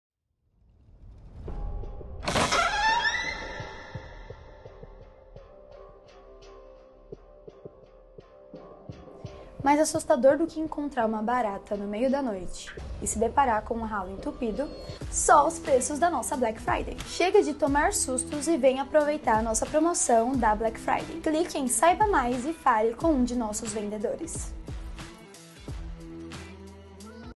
BLACK FRIDAY da Império: Prepare-se para Economizar!
Não é um filme, é a realidade! Prepare-se para a Black Friday na Império, onde os descontos vão te surpreender. Neste vídeo, revelaremos ofertas incríveis que você não vai querer perder. Economize enquanto protege seu espaço e cuida da sua saúde.